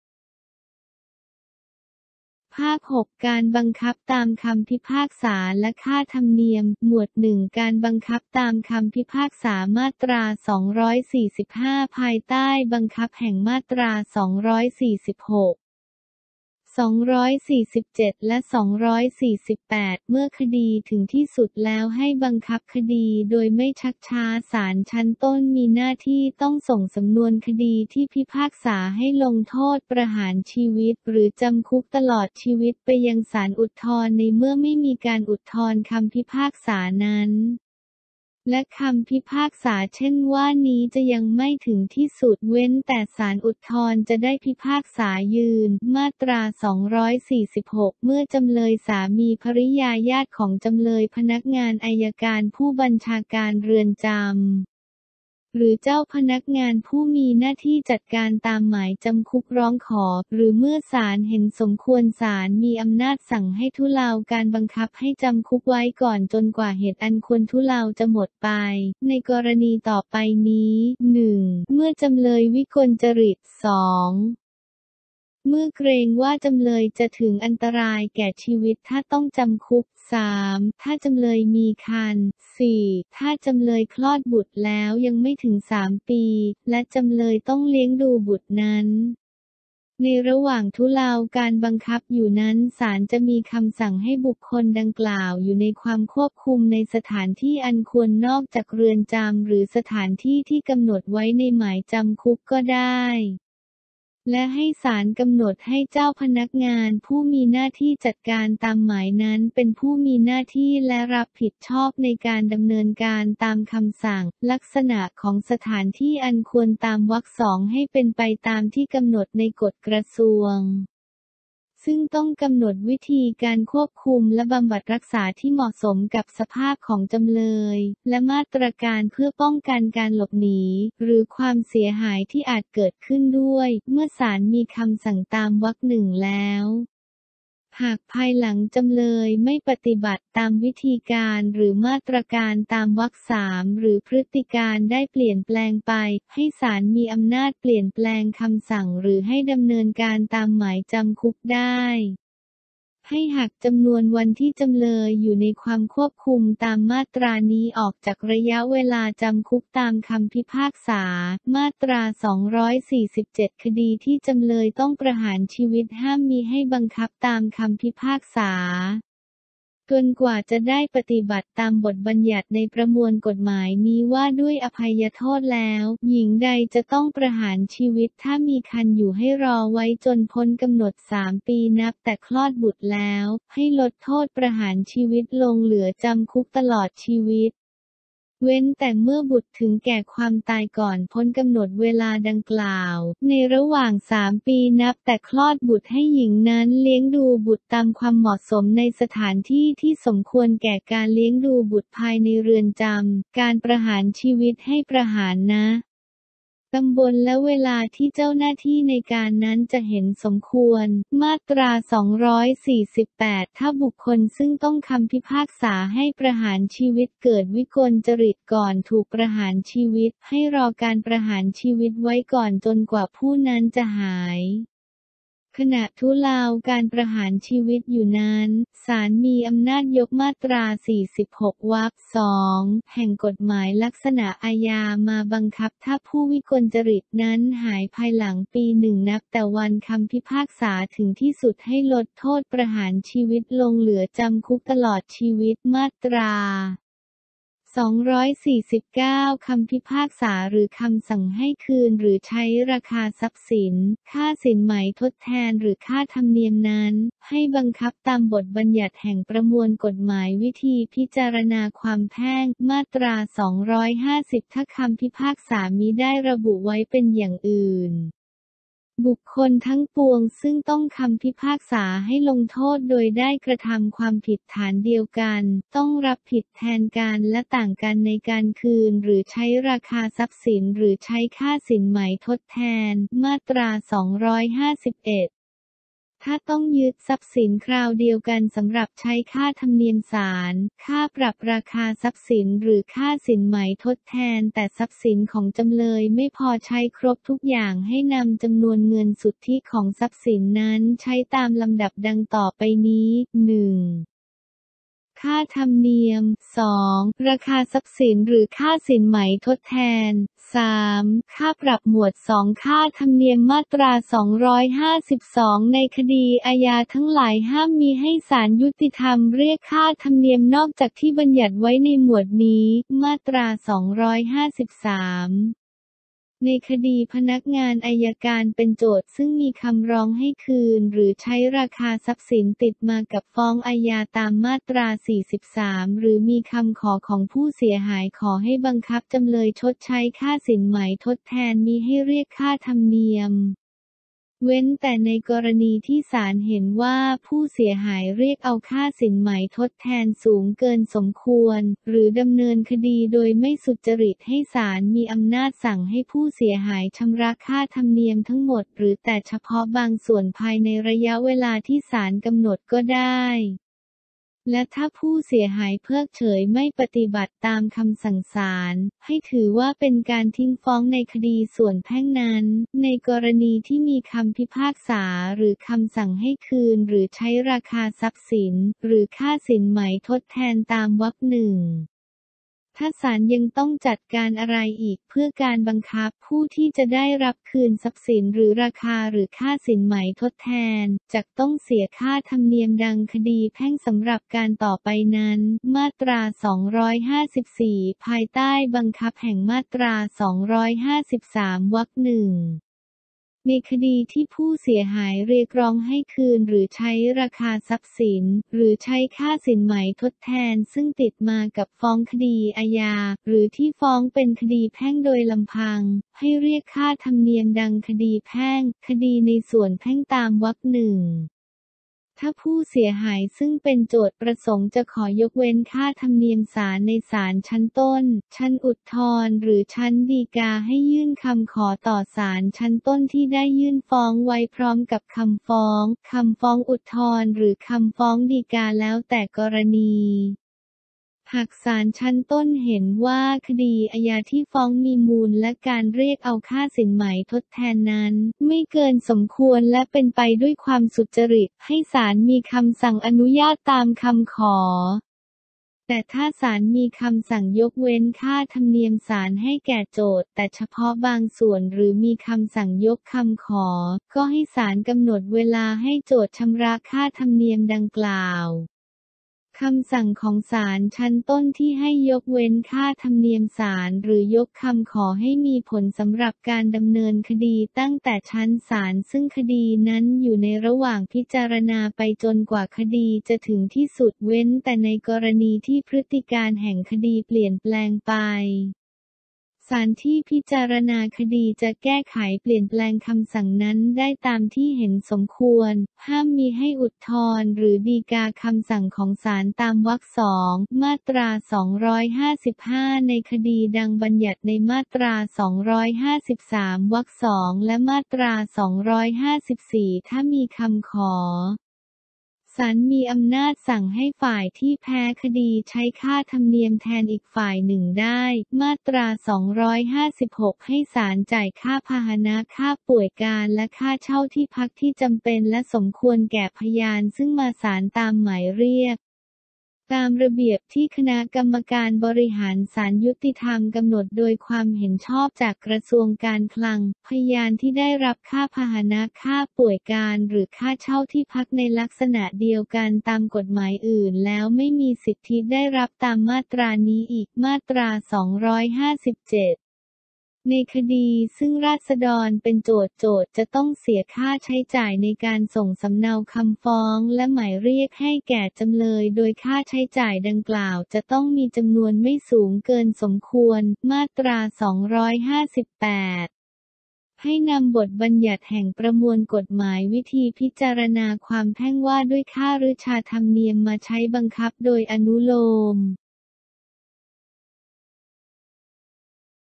เสียงอ่าน ป.วิ อาญา ม.254-258
ประมวลกฎหมายวิธีพิจารณาความอาญา
ภาค ๖ การบังคับตามคำพิพากษาและค่าธรรมเนียม
หมวด ๑ การบังคับตามคำพิพากษา  มาตรา ๒๔๕-๒๕๑
หมวด ๒ ค่าธรรมเนียม     มาตรา ๒๕๒-๒๕๘
อัพเดท ราชกิจจานุเบกษา เล่ม ๑๓๖/ตอนที่ ๓๔ ก/หน้า ๑๘/๒๐ มีนาคม ๒๕๖๒